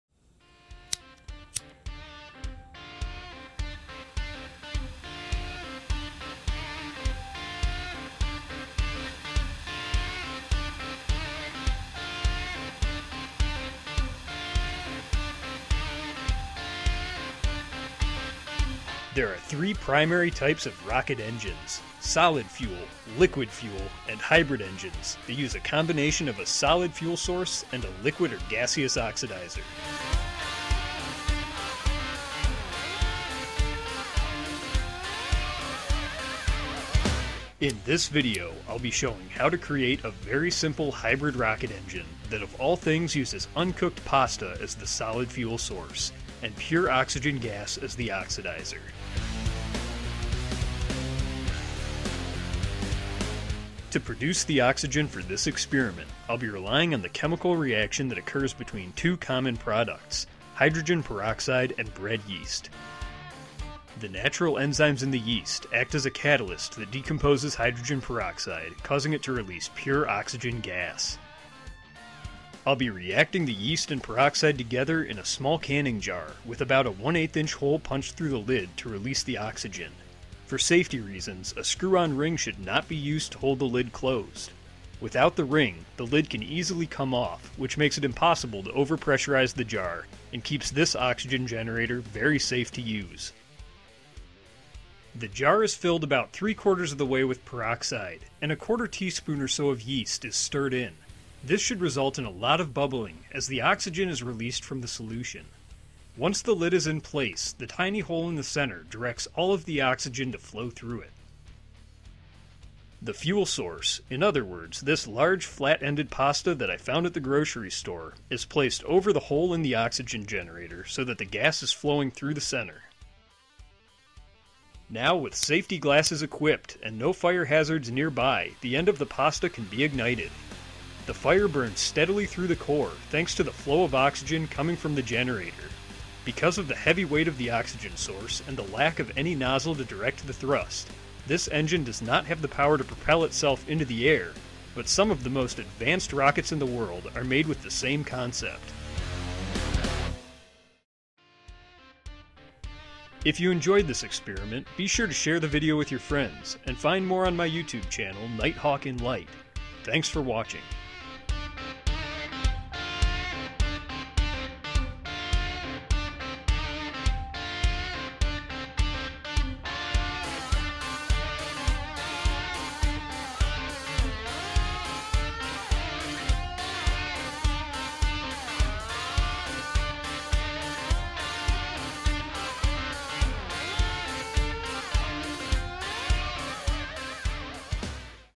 Make A Hybrid Rocket Engine With Pasta, Hydrogen Peroxide, And Yeast
Most simple rockets use a solid fuel engine which, once ignited, burns at a steady rate that cannot be controlled or stopped until the fuel is expended. Hybrid engines on the other hand can be turned on and off, and the thrust adjusted by varying how much oxidizer is fed into the ignition chamber to burn with the fuel. The simple hybrid engine made with this experiment lacks any valves that would be found in a full scale model to control the flow of oxygen, as it is the simplest form a hybrid engine could possibly take. You might call it a base model. If one were so inclined, all sorts of modifications could be made to this experiment to allow throttle control and even a nozzle.

For this project I used off the shelve 3% hydrogen peroxide solution, common bread yeast, and ziti macaroni noodles. This experiment should not be conducted without adult supervision.